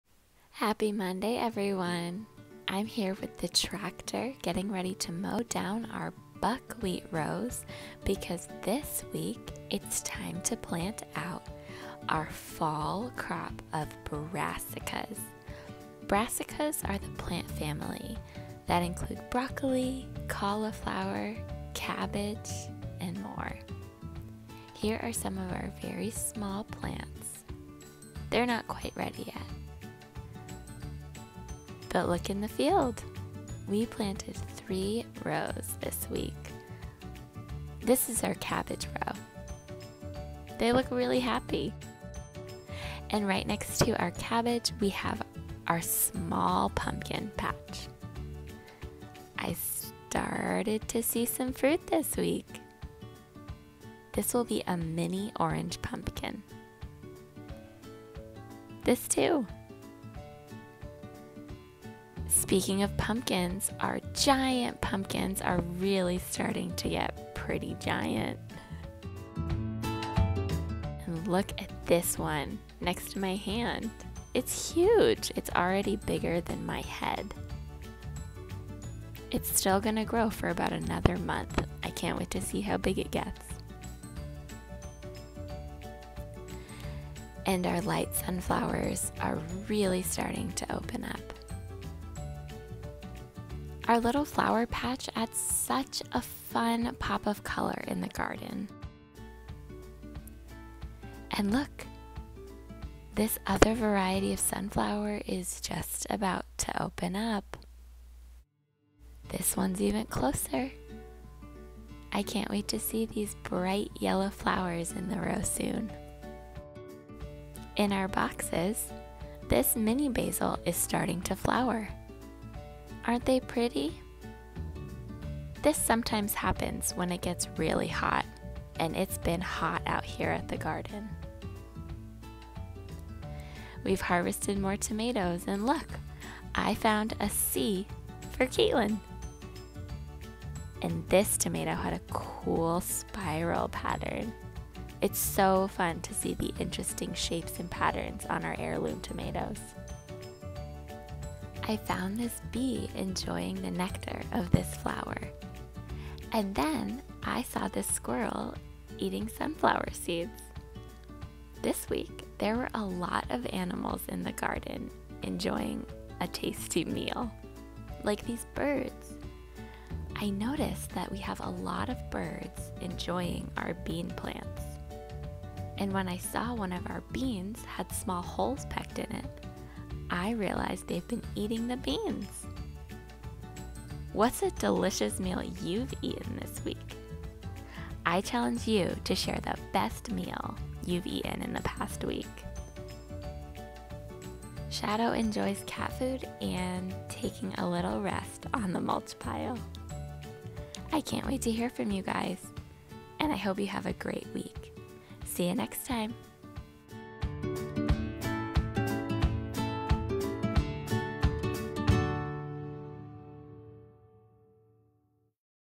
Garden Update (9/8) with Farmer Caitlin
It's fall planting time! Join Farmer Caitlin and Teacher Abby as they begin planting fall seedlings, check in on Shadow the cat, see how much the giant pumpkins have grown, and find a "C" shaped tomato! Watch the video to see who's been dining on the beans on our bean plants, and then share with us a delicious meal you had last week.

More Online Activity videos designed for adults with intellectual and/or developmental disabilities